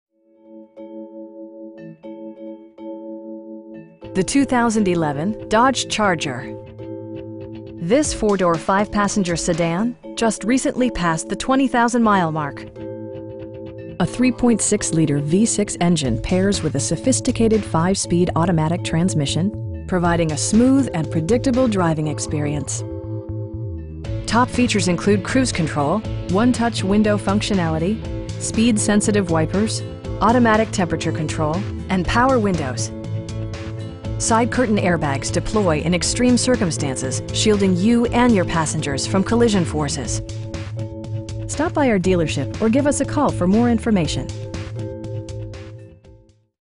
2011 Dodge Charger in Rincon, GA 31326
Southern Motors Rincon Select
5629 Highway 21 South
in Rincon, GA 31326

The 2011 Dodge Charger. This 4 door, 5 passenger sedan just recently passed the 20,000 mile mark! A 3.6 liter V-6 engine pairs with a sophisticated 5 speed automatic transmission, providing a smooth and predictable driving experience. Top features include cruise control, 1-touch window functionality, speed sensitive wipers, front bucket seats, automatic temperature control, tilt and telescoping steering wheel, power door mirrors, and power windows. Side curtain airbags deploy in extreme circumstances, shielding you and your passengers from collision forces. We pride ourselves in the quality that we offer on all of our vehicles. Stop by our dealership or give us a call for more information.